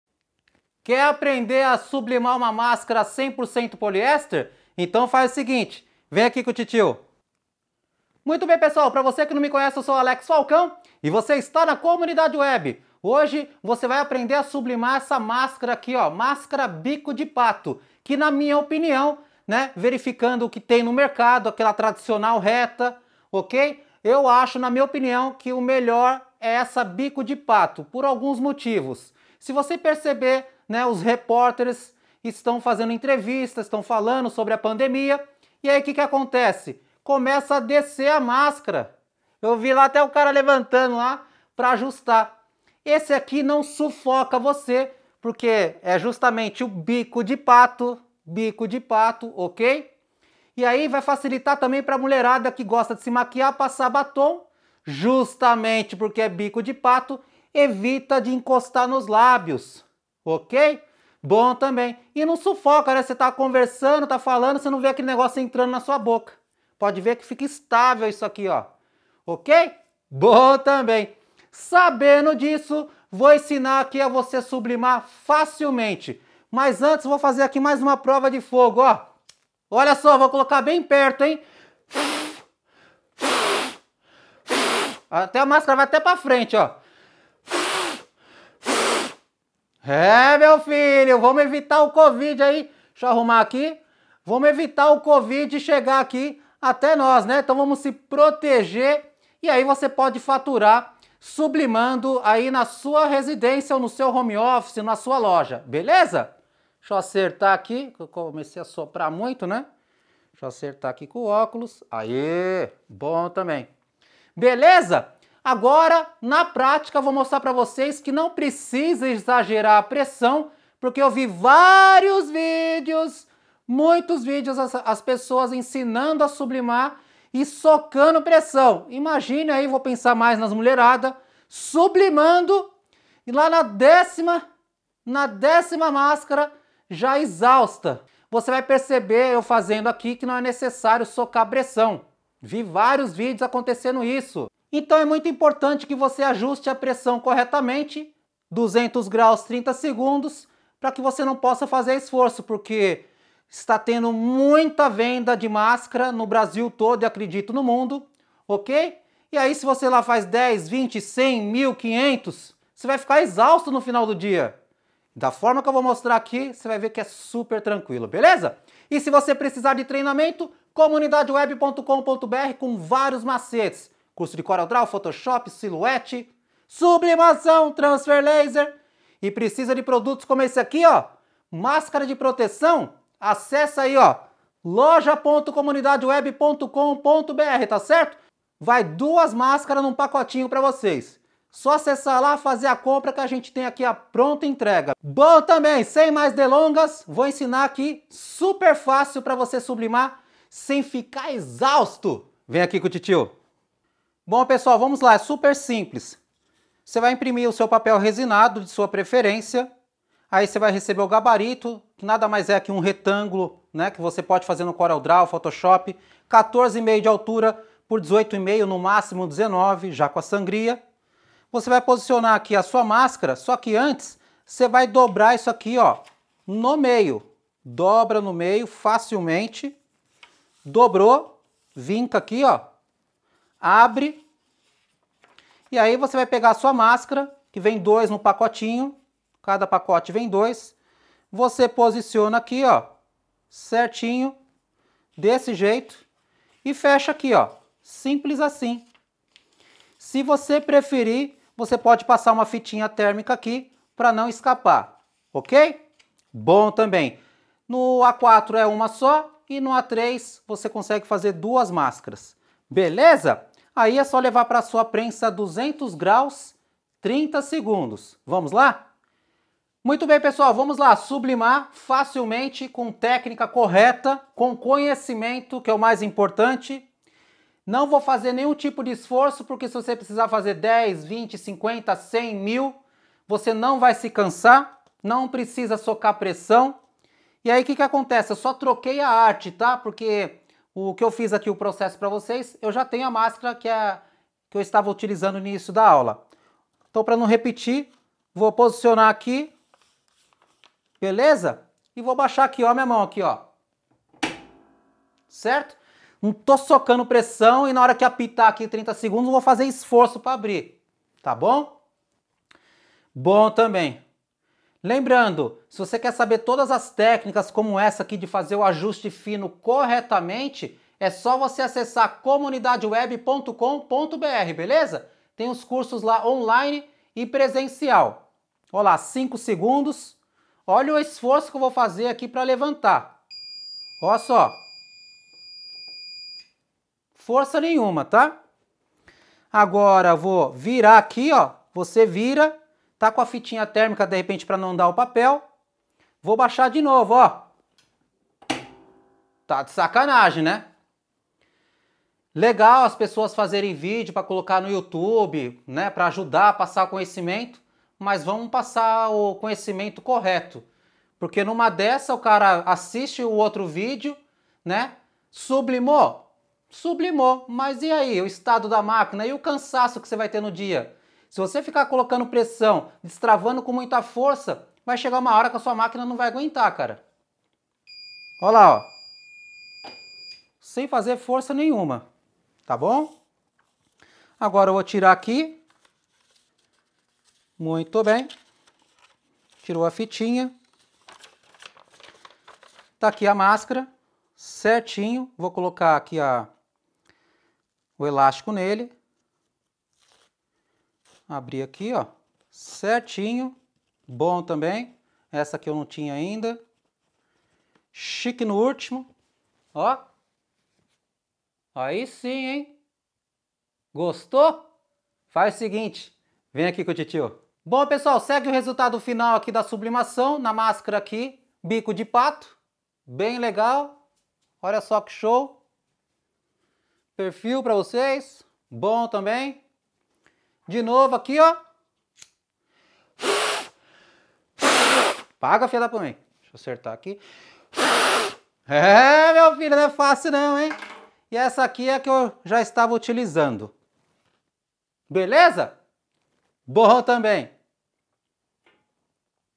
Como Estampar Máscara de Proteção para Sublimação Sem Fazer Esforço Físico.
Hoje você vai aprender estampar mascara de proteção para sublimação.
O mascara para sublimação tem tecido 100% poliéster de 200 gramas.
Você não vai precisar costurar pois já esta pronto para sublimar.
Você irá configurar sua prensa térmica em 200 graus / 30 segundos.
Depois vira o outro lado e bate mais 200 graus  / 30 segundos.